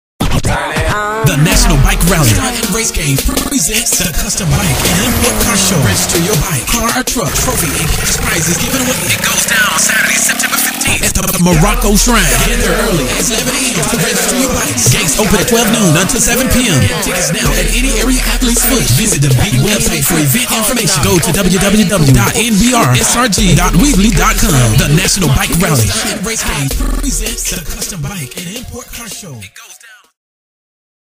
nbrsrg commercial
National Bike Rally Stunt and Race Games presents...
 NBRSR Custom Bike and Import Car Show

Sat September 15th 12-7p

 The Morocco Shrine Center

3800 Saint Johns Bluff Rd S 32224

-$10 General Admission/$15 at the door
-$15 advance registration/$20 day of to enter your ride
 -Registration begins Monday July 9th @ NBRSRG.WEEBLY.COM
-Register your ride the day of as early as 11 am, Registration closes at 3p
 -Trophy and Cash Prize Winners (cash prize to be determined)

ALL DAY FAMILY FESTIVAL EVENT (INDOOR/OUTDOOR)
TICKET LOCATIONS ALL JAX ATHLETE'S FOOT LOCATIONS AND 904 FASHIONS
$10 ONLINE TICKETS AT NBRSRG.WEEBLY.COM/$15 AT THE DOOR
 FAVORITE FOODS-VENDORS MARKET-BOUNCE HOUSES-GAMES AND PRIZES -DJ SPINNING ALL DAY-904 BOYS BIKE STUNT TEAM - BIKE SAFETY WORKSHOP

CASH BAR-APPAREL and sneaker buy, sell, TRADE-HOTTEST FALL FASHION RUNWAY SHOW

 DJ DES and DJ HYDROMATIC along withYOUR 93.3 THE BEAT VAN AND PERSONALITIES

VISIT NBRSRG.WEEBLY.COM FOR MORE EVENT INFORMATION
 SECURITY ONSITE-DRUG FREE EVENT-FREE PARKING

We are happy to announce our partnership with the Jacksonville Giants to help promote and bring awareness to BULLYING in Schools